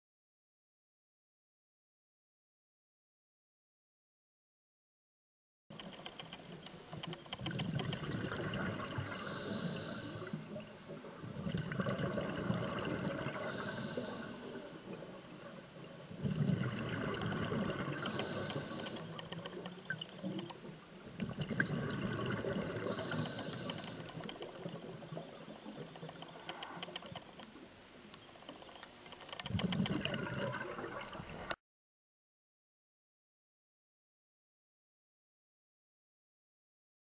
Orange Sea Pen (Ptilosarcus gurneyi)
An Orange Sea Pen filmed off South Pender Island - Gowlland Beach, BC.
Equipment - Olympus XZ-1 in PT-050 housing.